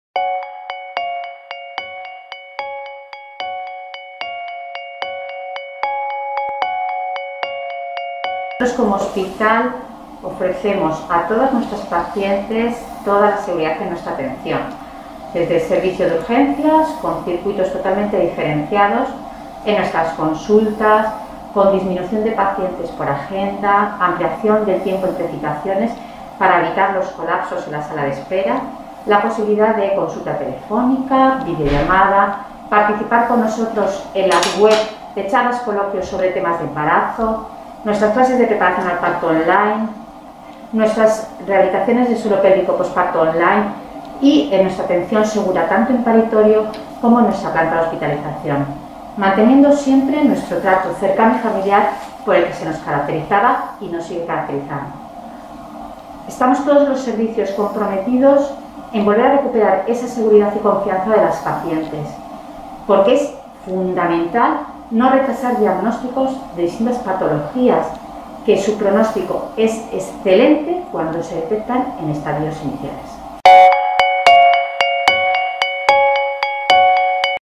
Quirónsalud Sur, hospital protegido frente a la COVID-19 en el embarazo y el parto
La Dra. Carmen Oteo nos relata las medidas adoptadas para atender el embarazo y el parto con seguridad frenta a la COVID-19.